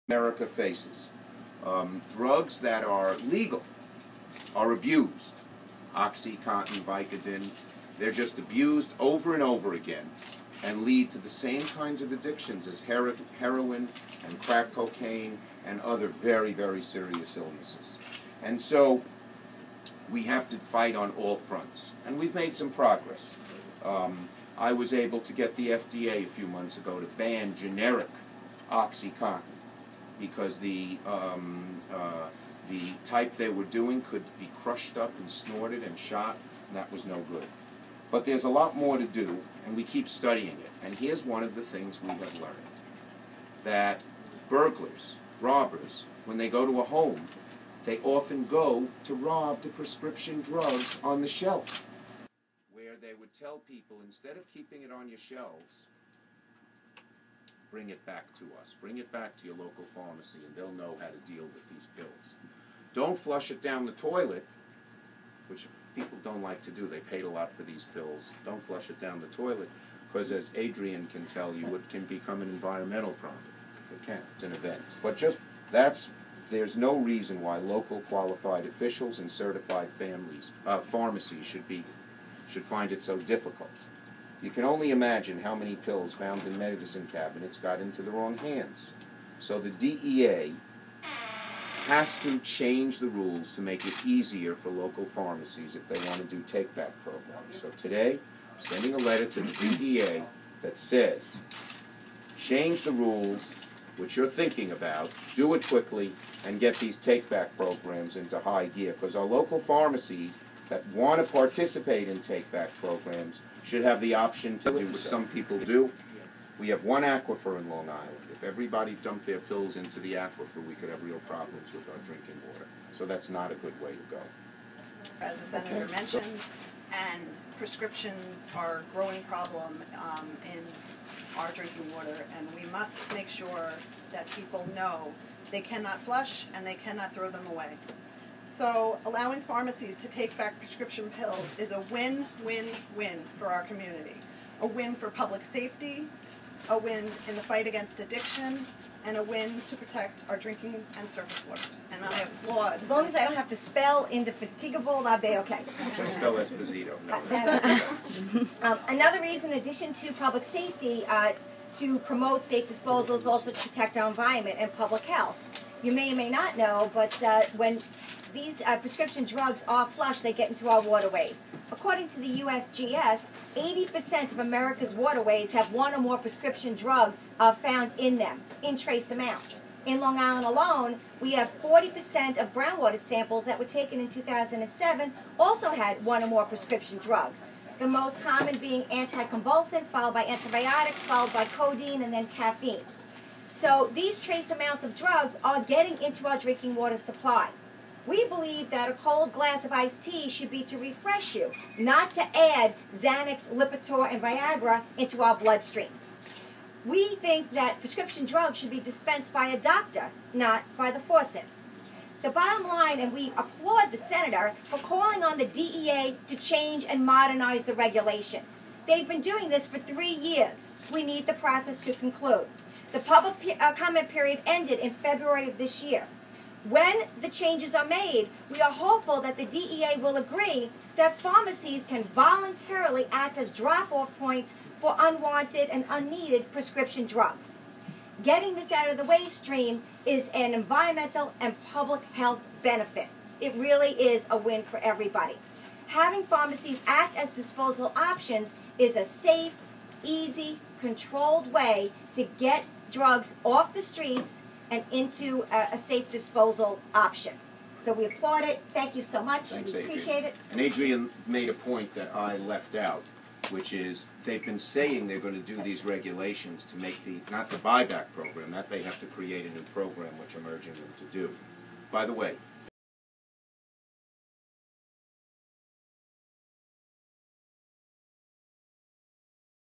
Senator Schumer press conference on safe disposal of pharmaceutical drugs made with Videoshop
CCE's Adrienne Esposito participated in Senator Schumer's press event on the safe disposal of pharmaceutical drugs.